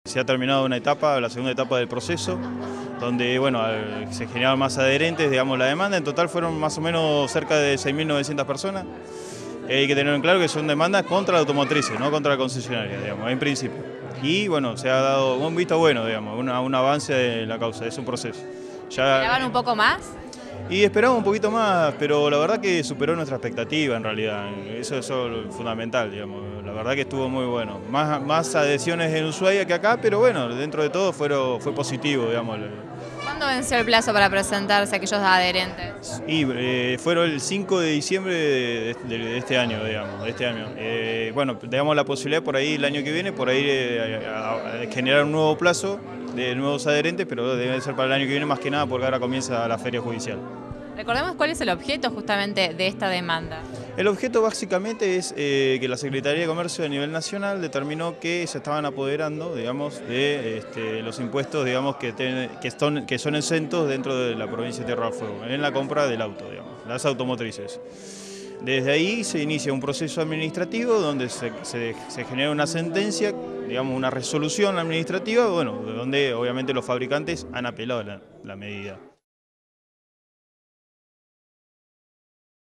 Demanda colectiva contra automotrices
El abogado Renán Uribe se refirió a la finalización de la segunda etapa de demandas colectivas, presentada contra las concesionarias de automóviles por el sobre precio en las ventas.